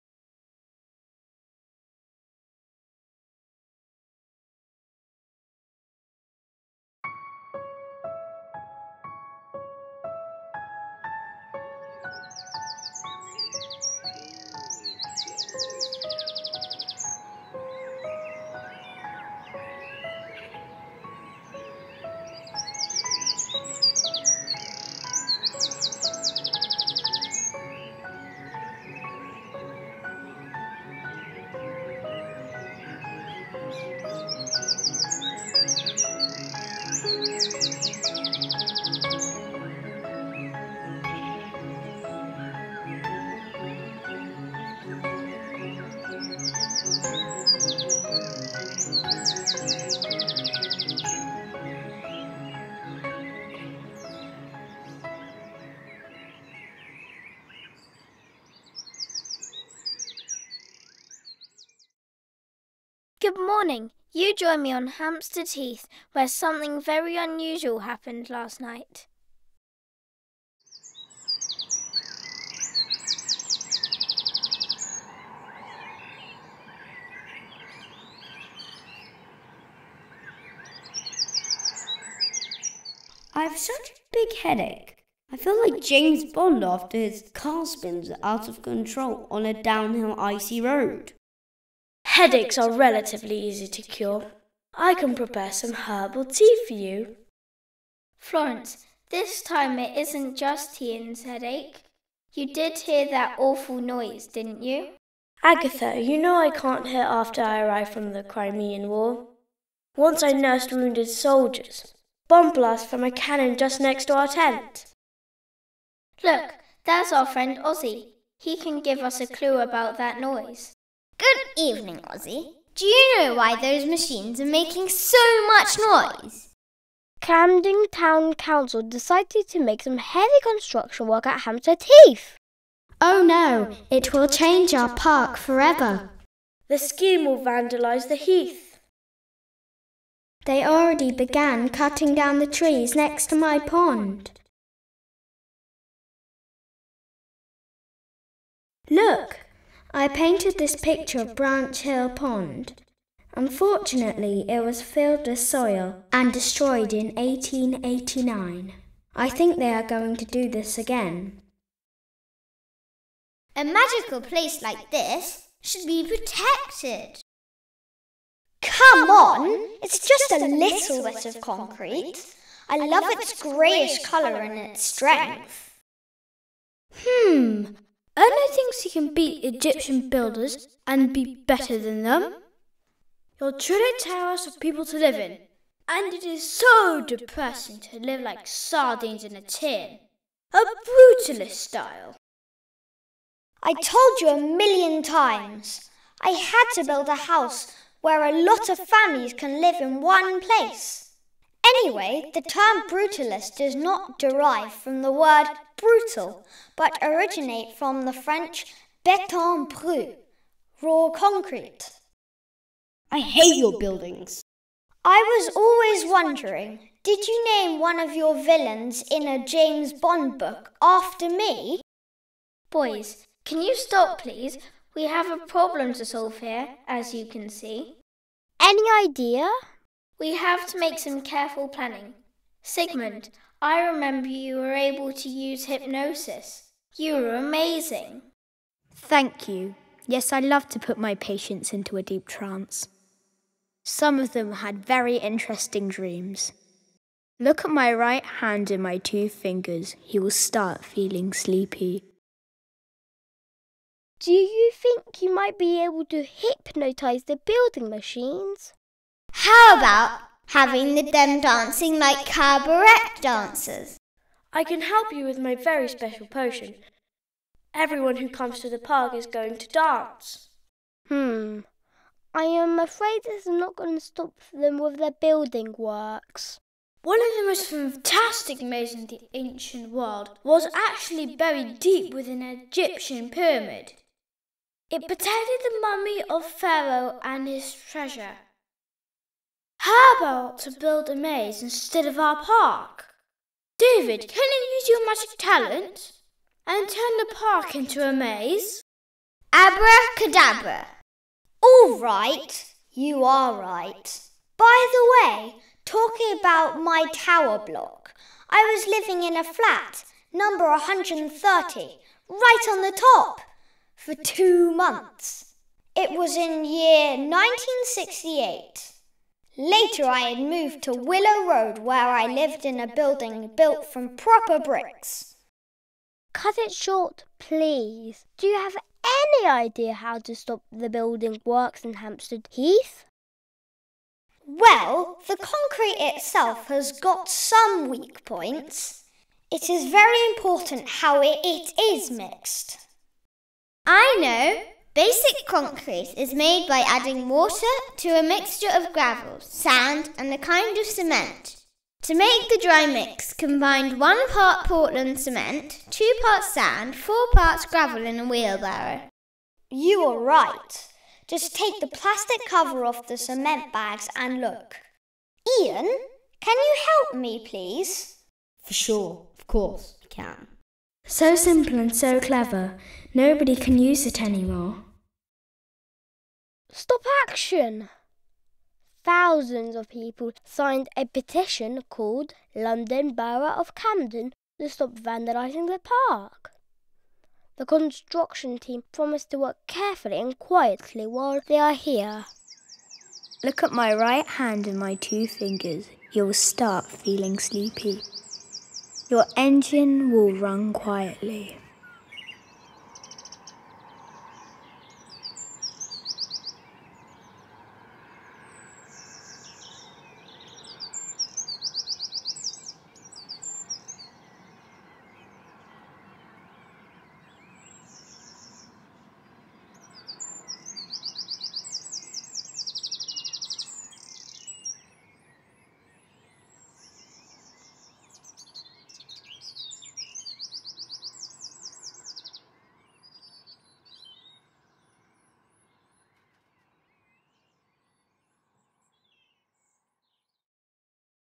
The Story of Hampstead Heath
Directed by Dora Martinkova and drawn and spoken by the children of Maple Walk School. (2015)